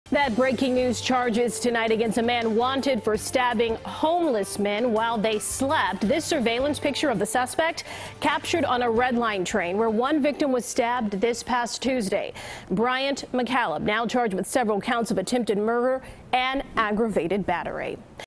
Bryant McCalip Accused Of Stabbing Homeless Men As They Slept
Bryant McCalip, 28, of Chicago, is charged with three felony counts of aggravated battery and three felony counts of attempted first degree murder. McCalip was arrested Thursday just before 2 p.m.